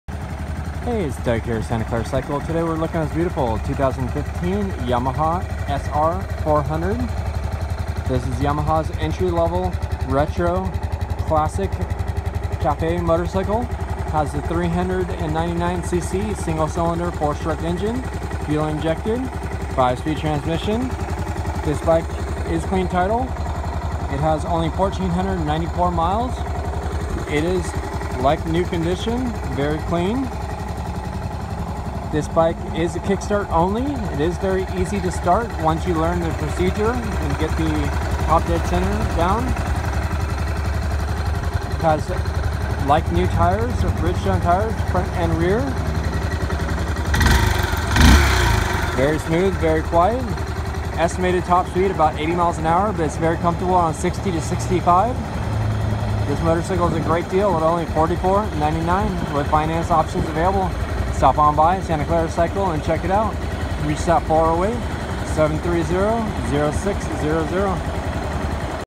2015 Yamaha SR400 .... Clean Classic Design w Low Miles!
15 Yamaha SR400
*clean CA title
*1948 only miles!
*399cc fuel injected single cylinder four stroke engine, 5 speed transmission
*classic style with modern brakes and fuel injection
*looks and runs great!
This super SR400 is offered @ only $4499 w financing options available w rates starting @ 0% apr for 12 months! w finance options available with rates starting @ 0% apr for 12 months!

You can see this motorcycle and many more @ Santa Clara Cycle ... SF South Bay Area's friendly used moto dealer!